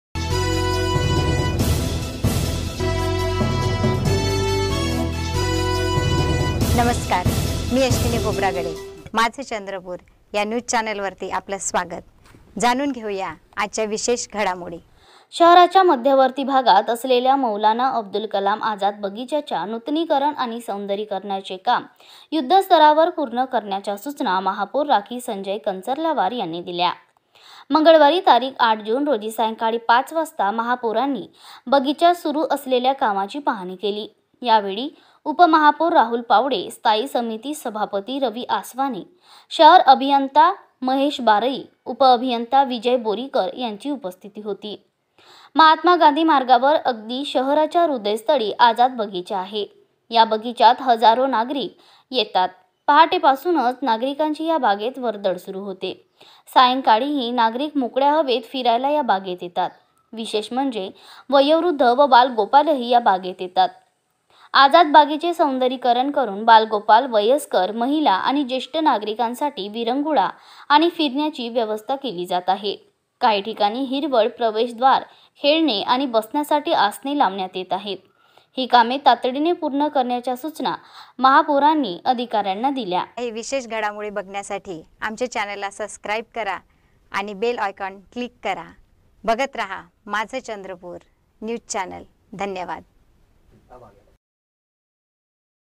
आझाद बगिच्याचे सौंदर्यीकरण तातडीने पूर्ण करा महापौर राखी संजय कंचर्लावार यांचे निर्देश
माझं चंद्रपूर न्यूज